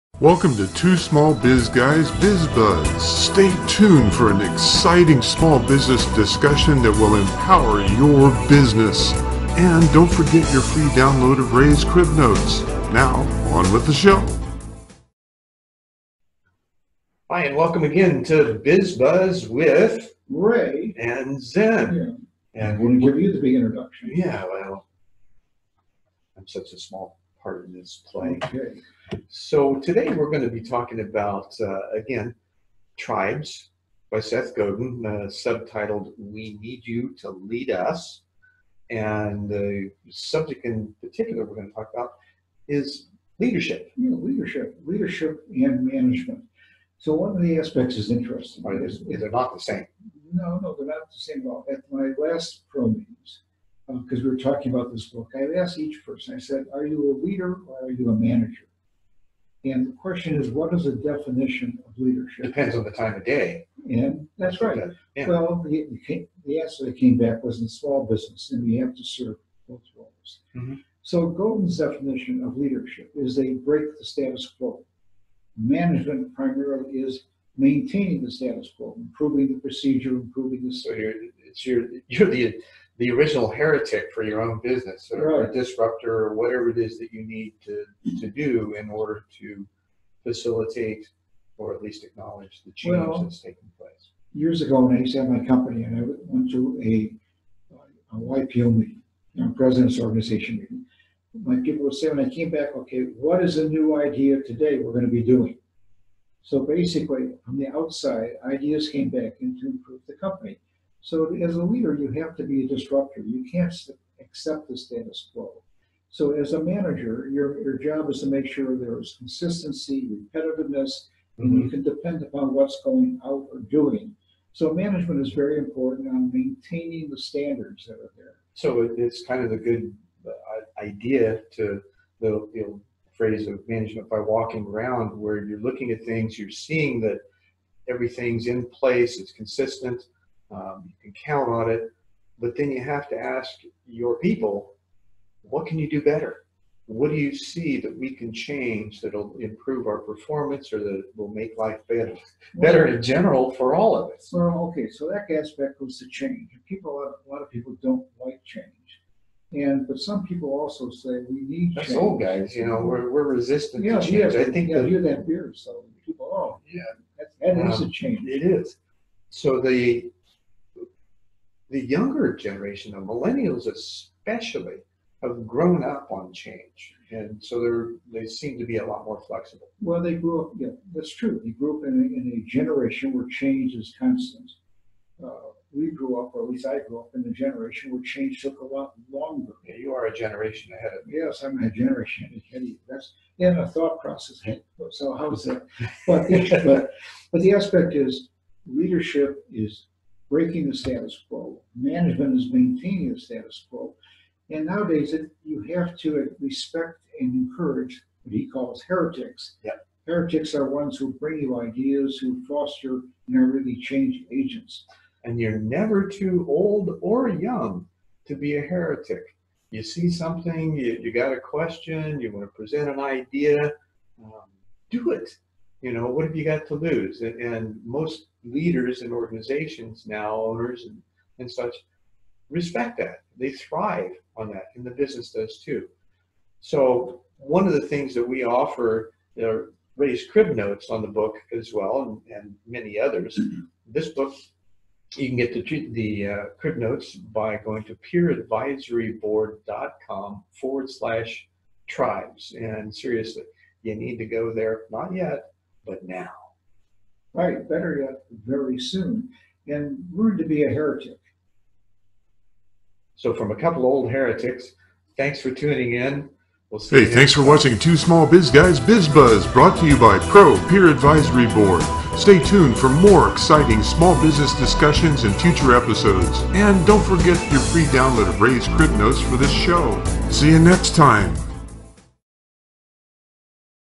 Biz Buzz - Tribes part 3 - Leadership is not Management
Leadership is not Management: Management is about manipulating resources to get a known job done. Managers  manage  a  process  they’ve seen before, and they react to the outside  world  striving  to make that process as fast and as cheap as possible.